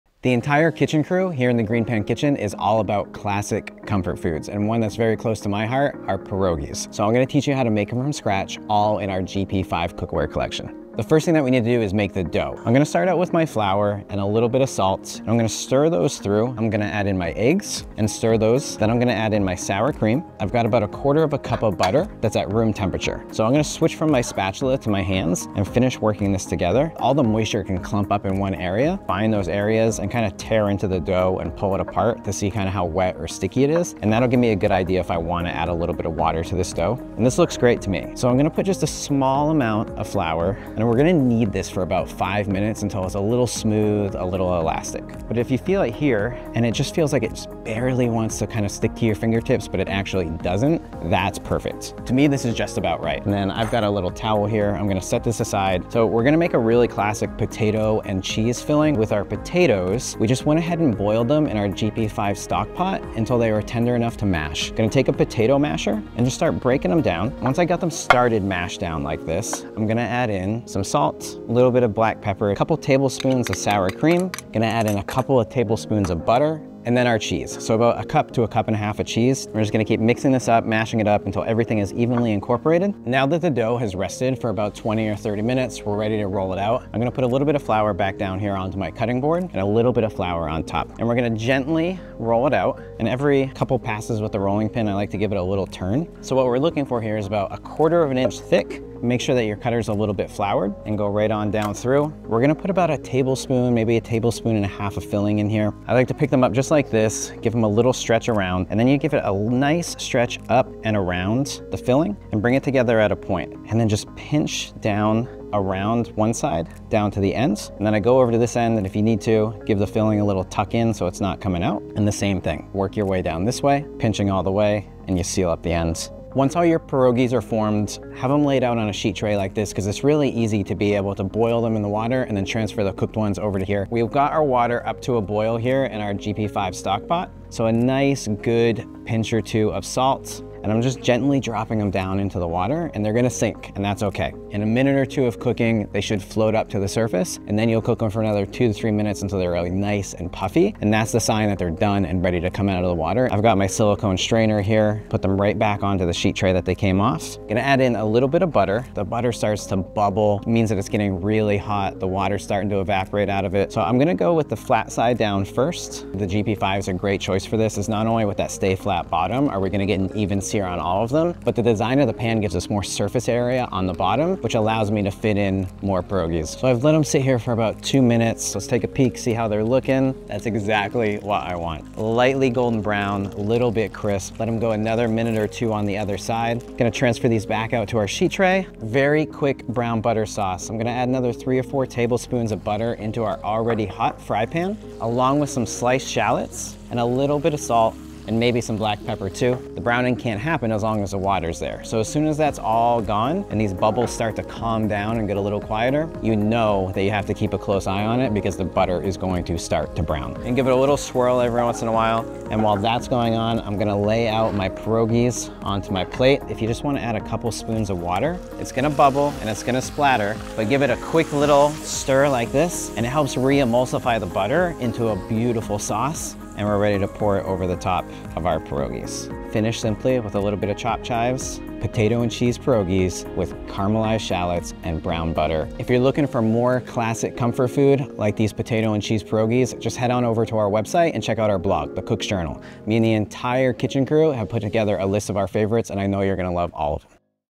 Potato and Cheese Pierogi with Brown Butter Sauce - GreenPan
A classic comfort food 🥟✨ These homemade pierogi, filled with creamy mashed potatoes and cheddar cheese, are seared to golden perfection and served with a rich brown butter and shallot sauce. The perfect side dish, main, or snack for spud-lovers! ⁣